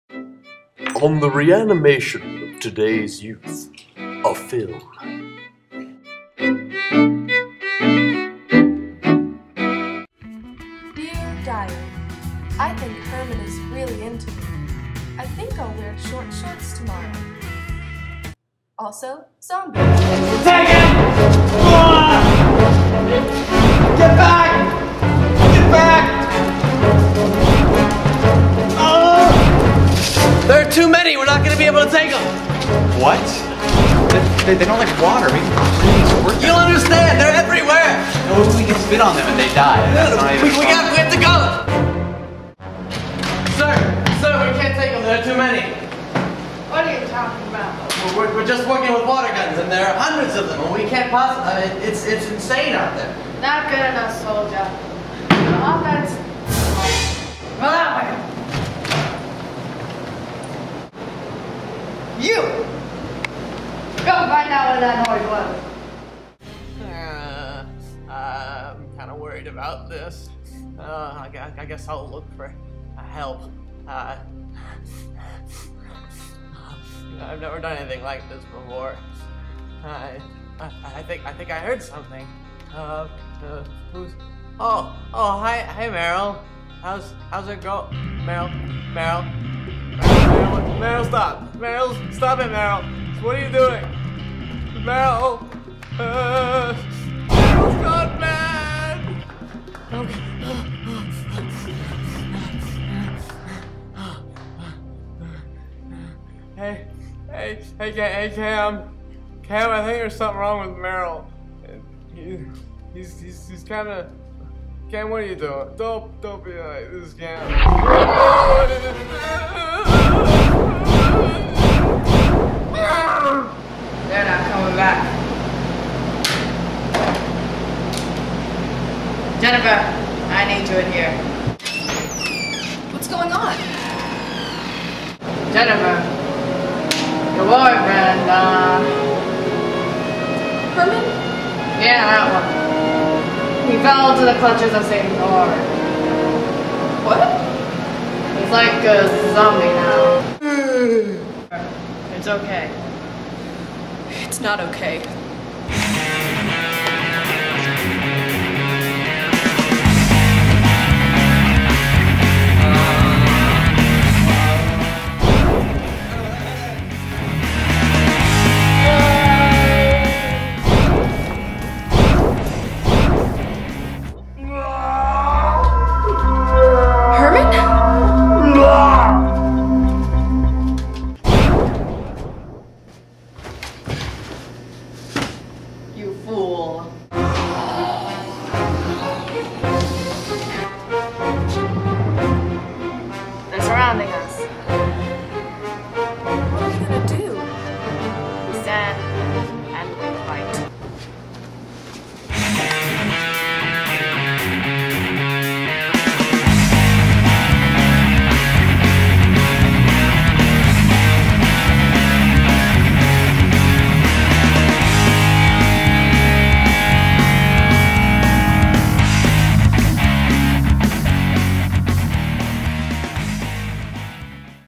On the Reanimation of Today's Youth- A Film
A film for the final project in Celluloid Visions by Group Orange